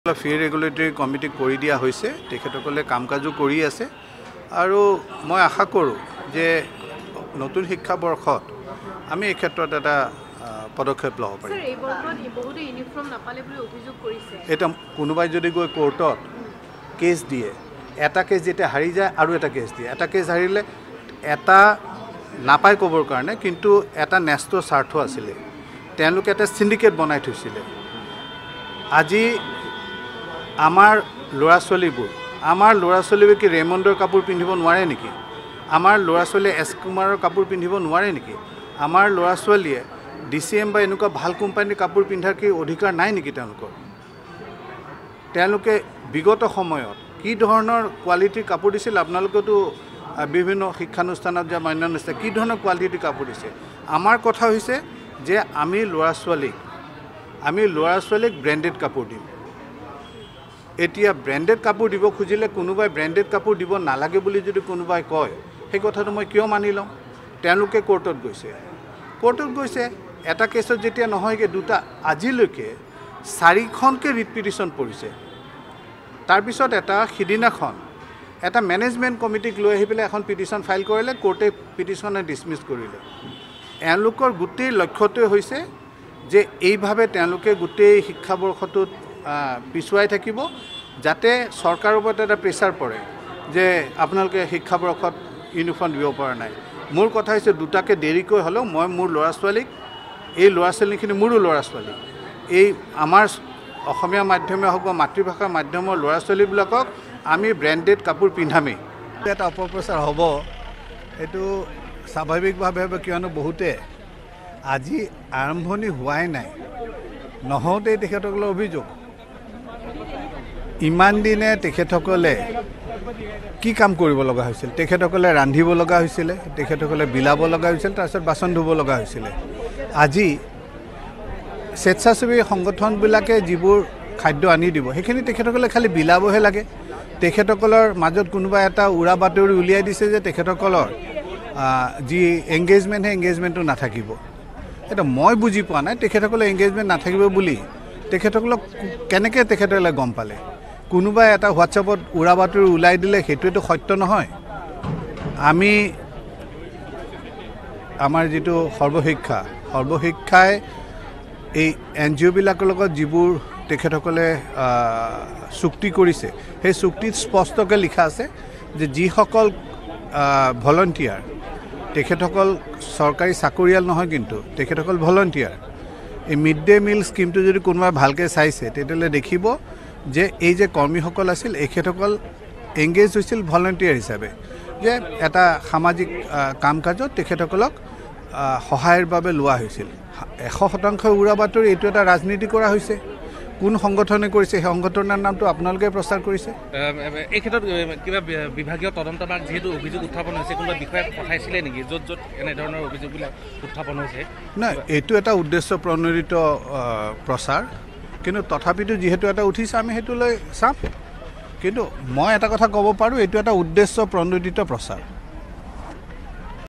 প্ৰসংগ : ছাত্ৰ-ছাত্ৰীৰ বিনামূলীয়া স্কুল ইউনিফৰ্ম | Minister Bhattacharya to Give Branded Uniform
ৰাজ্যৰ কণকণ ছাত্ৰ-ছাত্ৰীয়ে স্কুল ইউনিফৰ্ম এতিয়ালৈকে লাভ নকৰা সন্দৰ্ভত প্ৰতিক্ৰিয়া প্ৰকাশ কৰিলে শিক্ষামন্ত্ৰী সিদ্ধাৰ্থ ভট্টাচাৰ্যই। আদালতৰ মজিয়াত স্কুল ইউনিফৰ্মৰ বিষয়টো থকাৰ বাবেই চৰকাৰে দিব পৰা নাই বিনামূলীয়া ইউনিফৰ্ম। বিষয়টো সন্দৰ্ভত আদালতত চলি আছে চাৰিটা গোচৰ। এটা শক্তিশালী ছিণ্ডিকেট চক্ৰই ছাত্ৰ-ছাত্ৰীক ব্ৰেণ্ডেড কাপোৰ দিয়াৰ ক্ষেত্ৰত হেঙাৰ হৈ থিয় দিছে চৰকাৰৰ সন্মুখত। শিক্ষামন্ত্ৰীৰ প্ৰশ্ন- আমাৰ ল’ৰা-ছোৱালীয়ে কিয় ব্ৰেণ্ডেড কাপোৰ পিন্ধিব নোৱাৰিব।